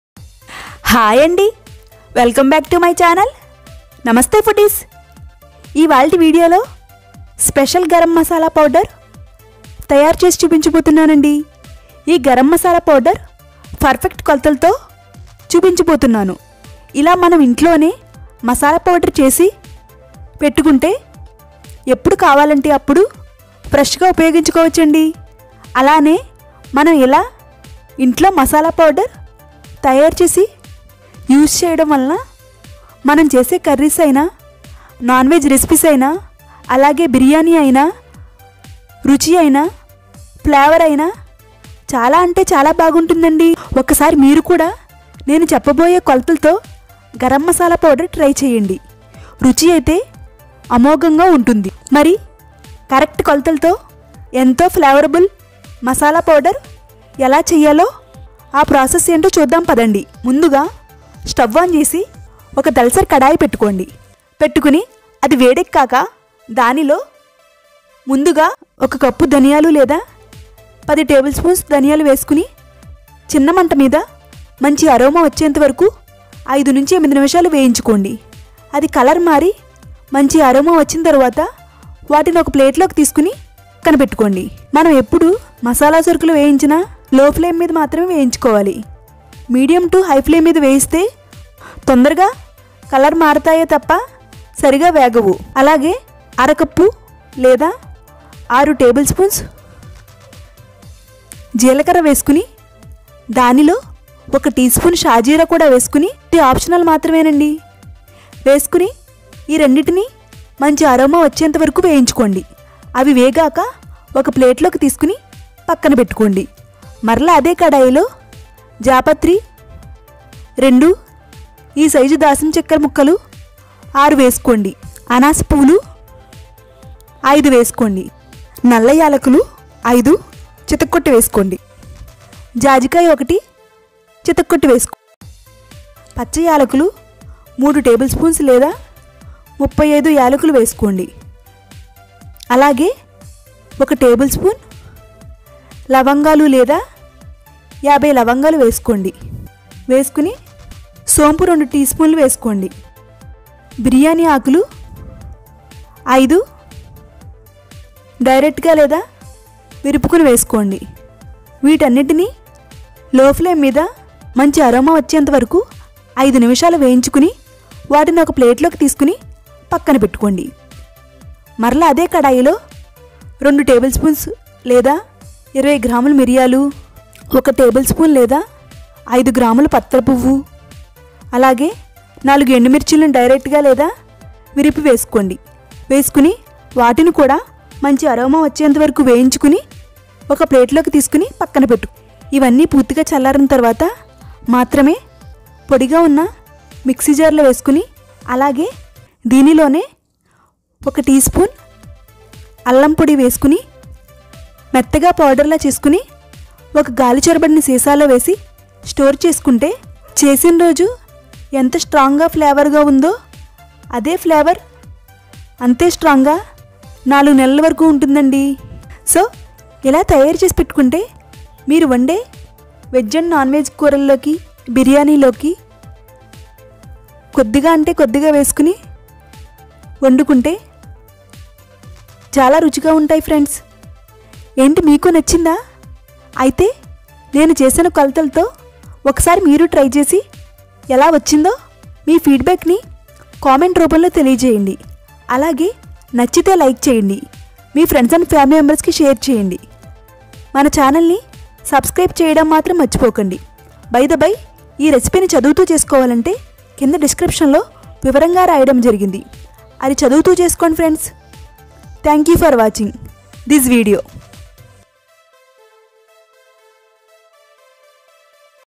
Homemade garam masala powder||veg & non veg కర్రీస్ లోకి👉 పక్కా కొలతలతో చేసి వేస్తే 'రుచి అమోఘం
Homemade Garam Masala recipe

Heavy bottom pan
Coriander seeds  - 1 cup/10 tablespoons
Roast low flame
Turns aromatic
Keep aside

Black Cumin seeds - half teaspoon/6 tablespoons
Caraway (Shah jeera)- one teaspoon (optional)
Roast low flame
Turns aromatic
Keep aside

Mace - 2
Cinnamom - 4 inches/6 pieces
Nutmeg - 1
Green cardamom - 3 teaspoons/35 pieces
Clove - 1 tablespoon/ 50 pieces
Finnel - 2 teaspoons
Bay leaf 🍃- 5
Roast low flame
Turns aromatic
Keep aside

Black Paper - 2 tablespoons / 20 grams
Stone flower - 1 tablespoon/5 grams/5 pieces

Cool completely
Transfer - To mixi jar
Ginger powder one teaspoon
Blend - smooth powder
Homemade garam masala powder ready

హోమ్ మేడ్ గరంమసాలా పౌడర్ కావలసినవి : -
హోంమేడ్ గరం మసాలా పౌడర్ తయారీ విధానం : -
1  ముందుగా 'స్టవ్ ఆన్ చేసి '  ఒక తలసరి కడాయి పెట్టుకుని 'అది వేడెక్కాక ఒక కప్పు లేదా 10 టేబుల్స్ ధనియాలు వేసుకుని చిన్న మంట మీద అ మంచి అరోమా వచ్చేంతవరకు వేయించుకుని 'అవి కలర్ మరి మంచి ఆరోమా వచ్చిన  తరువాత 'ఒక ప్లేట్లోకి తీసుకుని పక్కన పెట్టుకోవాలి .
2 మళ్లీ అదే కడాయిలో అర కప్పు లేదా 6 టేబుల్ స్పూన్లు జీలకర్ర వేసుకుని దానిలో ఒక టీ స్పూన్ షాజీరా వేసుకుని ఇది ఆప్షనల్ మాత్రమే 'కలర్ మరి ఇ మంచి ఆరోమా వచ్చేంతవరకు వేయించుకుని ఒక ప్లేట్లోకి తీసుకుని పక్కన పెట్టుకోవాలి .
3  మరలా అదే కడాయిలో రెండు జాపత్రి 'నాలుగు ఇంచులు దాసించెక్క 'అనాసపువ్వు 5 'జాజికాయ ఒకటి చితక్కొట్టి వేసుకోండి .పచ్చ యాలకులు మూడు టేబుల్ స్పూన్స్ లేదా35 యాలకులు 'ఒక టేబుల్ స్పూన్ లవంగాలు లేదా 50 లవంగాలు,సోంపు రెండు టీ స్పూన్లు 'బిర్యానీ ఆకు లు 5 'వీటన్నిటిని లో ప్లే మీద మంచి ఆరోమా వచ్చేంతవరకు వేయించుకుని ఐదు నిమిషాలు ఒక ప్లేట్లోకి తీసుకుని పక్కన పెట్టుకోవాలి .
4  మరలా అదే కడాయిలో రెండు టేబుల్ స్పూన్స్ మిరియాలు లేదా 20 గ్రాములు ' పత్తర్‌ పువ్వు ఒక టేబుల్ స్పూన్ లేదా ఐదు గ్రాములు 'వేసుకుని వాటిని కూడా మంచి ఆరోమా వచ్చేంతవరకు లో ప్లే మీద వేయించుకుని ఒక ప్లేట్ లోకి తీసుకొని పక్కన పెట్టుకోవాలి ..
 5  ఇవన్నీ పూర్తిగా చల్లారిన తర్వాత మాత్రమే పొడిగా నాకు మిక్సీ జార్ లో వేసుకుని 'దీనిలోనే ఒక టీస్పూన్ అల్లం పొడి వేసుకుని మెత్తగా పొడిలా చేసుకుని 'ఒక గాలి చొరబడని సీసాలో నిల్వ చేసుకుంటే నాలుగు నెలలు నిల్వ ఉంటుంది .
6 ఈ మసాలా పౌడర్ మీరు తయారు చేసే 'వెజ్ నాన్ వెజ్ కర్రీ లోకి 'బిర్యానీలోకి వేస్తే చాలా రుచిగా ఉంటుంది ఫ్రెండ్స్ '
7 ఈ రెసిపీ మీరు తప్పకుండ ట్రై చేయండి .ట్రై చేసి ఇ ఎలా వచ్చిందో కింద కామెంట్ చేయండి .